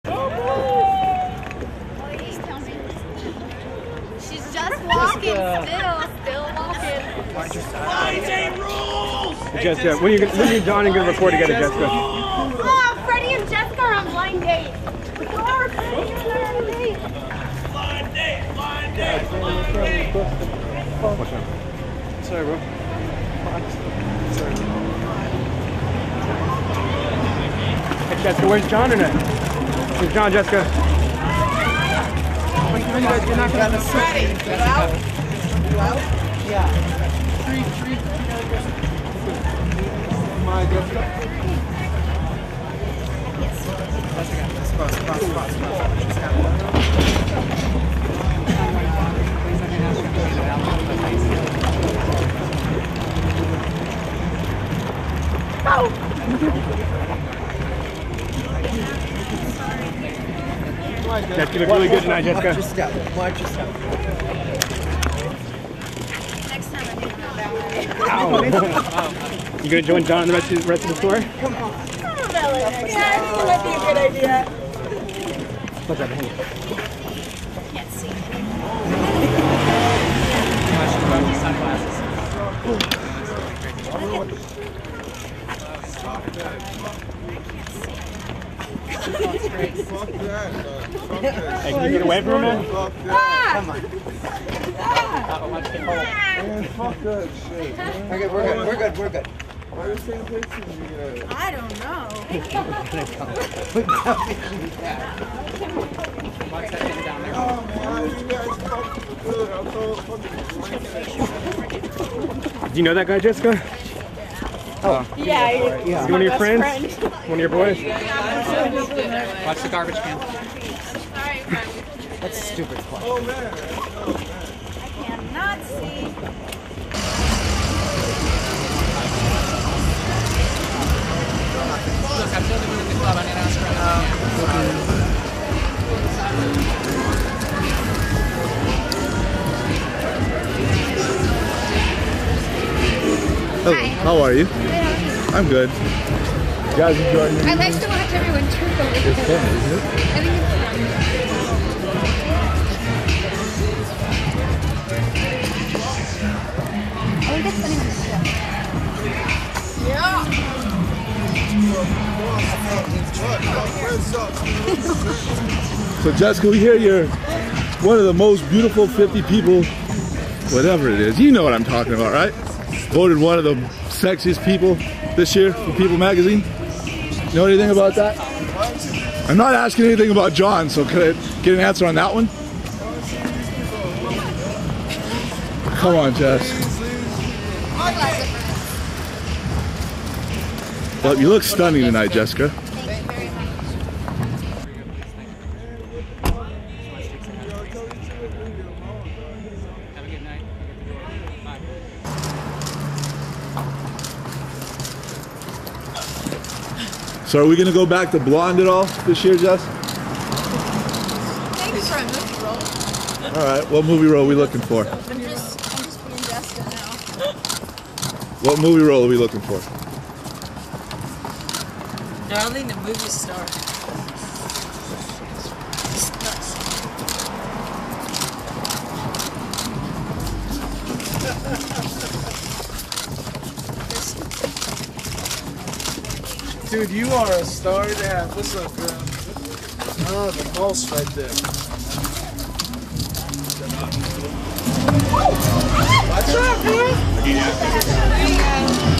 Jessica Simpson Greets The Crowd In Hollywood [2007]
April 16, 2007: Jessica Simpson wore a low-cut dress for an evening out on the town, attracting much attention to her natural endowments.  X17online.com